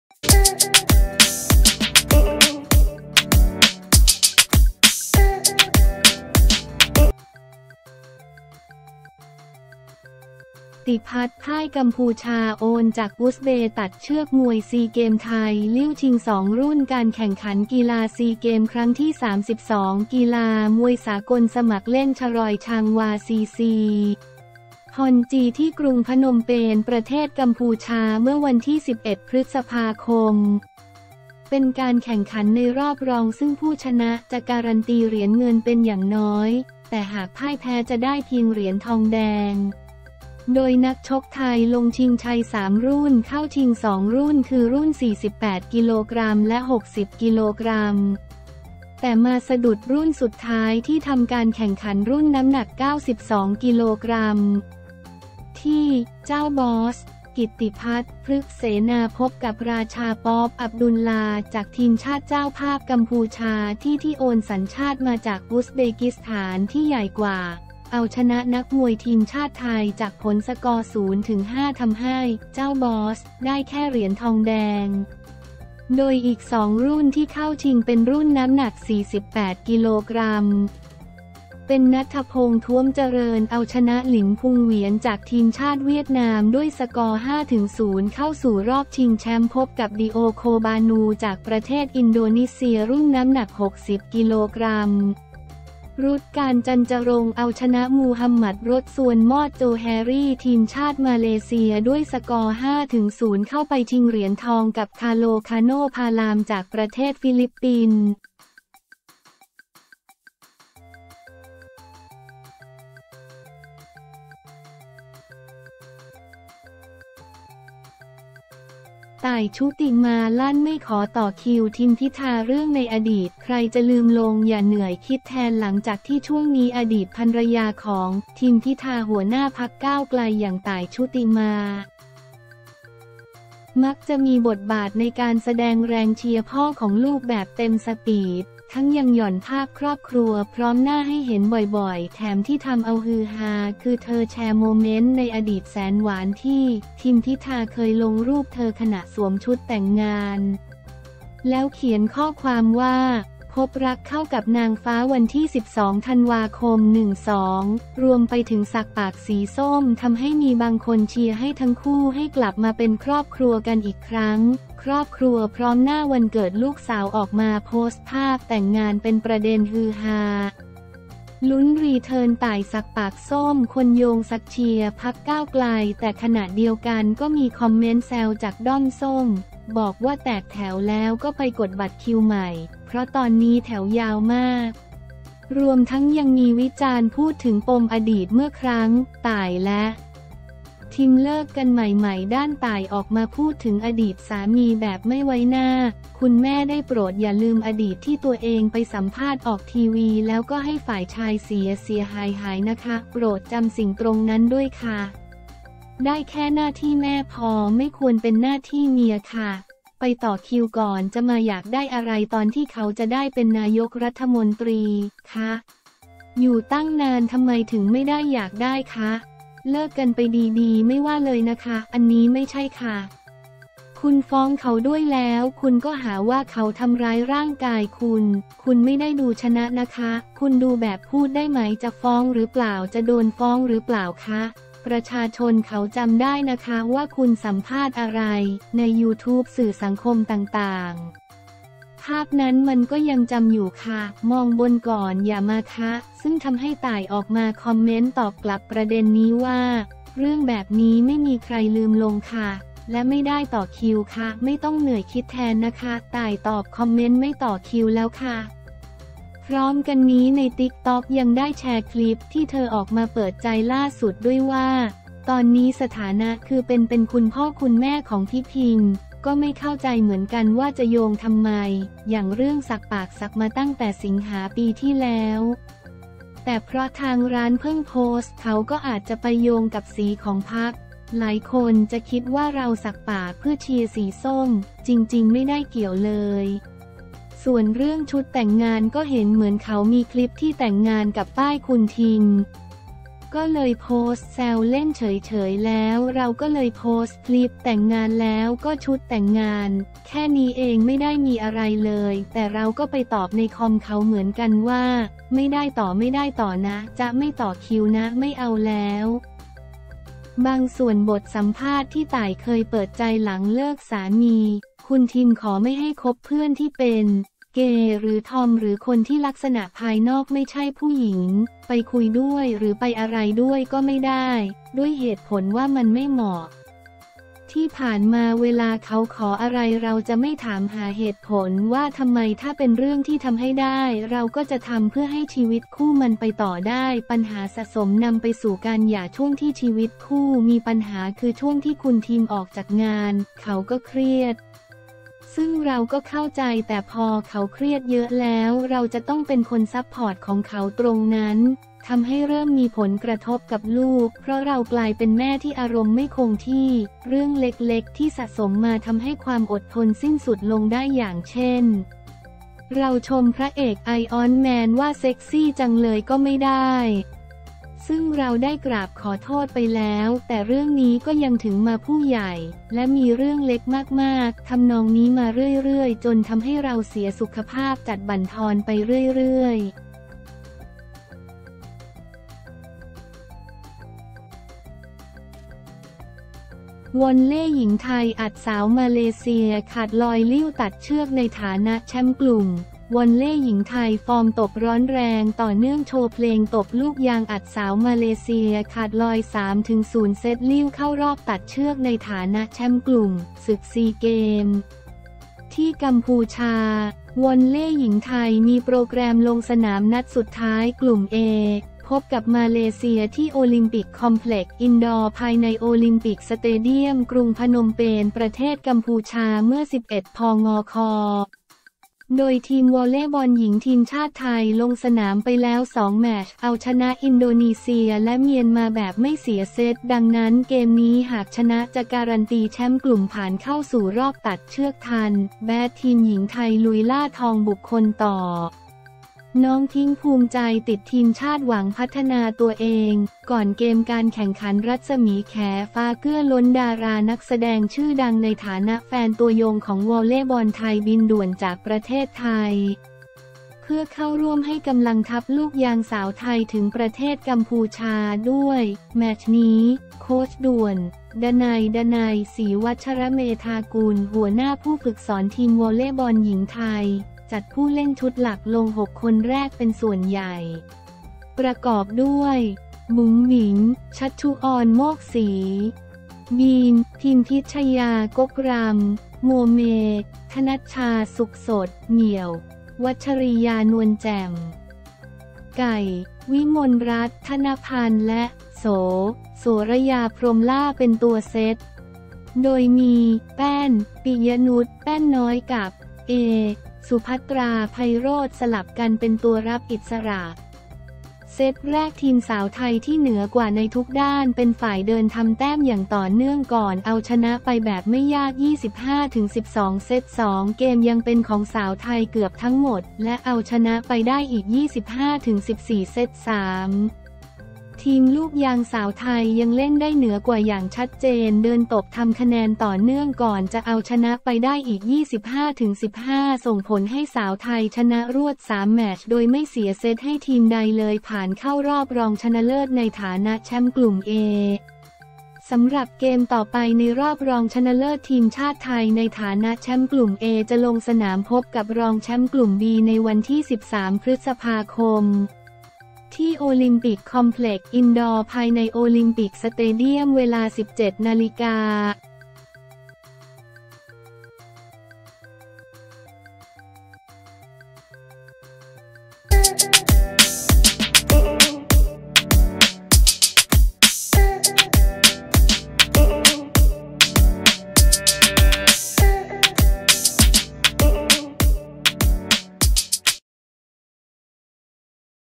กิตติภัฏ พ่าย กัมพูชา โอนจากอุซเบฯ ตัดเชือก มวย ซีเกมส์ ไทยลิ่วชิง 2 รุ่น
การแข่งขันกีฬา ซีเกมส์ ครั้งที่ 32 กีฬามวยสากลสมัครเล่น ชรอย ชางวาร์ ซีซี ฮอลล์ จี ที่กรุงพนมเปญ ประเทศกัมพูชา เมื่อ 11 พ.ค. เป็นการแข่งขันในรอบรองฯ ซึ่งผู้ชนะจะการันตีเหรียญเงินเป็นอย่างน้อย แต่หากพ่ายแพ้จะได้เพียงเหรียญทองแดง
โดย นักชกไทยลงชิงชัย 3 รุ่น เข้าชิง 2 รุ่น คือรุ่น 48 กก. และ 60 กก. แต่มาสะดุดรุ่นสุดท้ายที่ทำการแข่งขันรุ่นน้ำหนัก 92 กก. ที่ “เจ้าบอส” กิตติภัฎ พรึกเสนา พบกับ ราชาปอฟ อับดุลลา จากทีมชาติเจ้าภาพกัมพูชา ที่ที่โอนสัญชาติมาจากอุซเบกิสถานที่ใหญ่กว่า เอาชนะนักมวยทีมชาติไทยจากผลสกอร์ 0-5 ทำให้ “เจ้าบอส” ได้แค่เหรียญทองแดง
โดยอีก 2 รุ่นที่เข้าชิงฯ เป็น รุ่นน้ำหนัก 48 กก. เป็น ณัฐพงษ์ ท้วมเจริญ เอาชนะ หลิน พุง เหวียน จาก ทีมชาติเวียดนามด้วยสกอร์ 5-0 เข้าสู่รอบชิงแชมป์พบกับ ดิโอ โคบานู จากประเทศอินโดนิเซีย

รุ่นน้ำหนัก 60 กก. รุตชการญ์ จันทร์จรง เอาชนะ มูฮัมหมัด รดซวน มอด โจแฮรี่ ทีมชาติมาเลเซียด้วยสกอร์ 5-0 เข้าไปชิงเหรียญทองกับ คาร์โล คาโน พาลาม จากประเทศฟิลิปปินส์